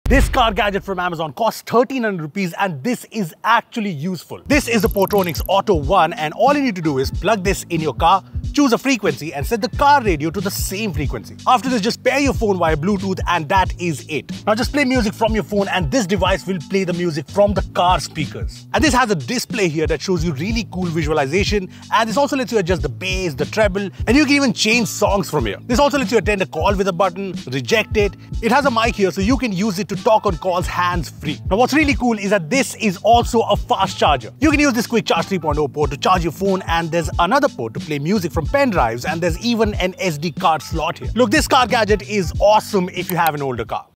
Super Useful Car Gadget from Amazon!
In this Short, we show you a ₹1,300 car gadget from Amazon that's really useful. We are talking about the Portronics Auto One, which works as Bluetooth streamer, charger and it has a really cool display.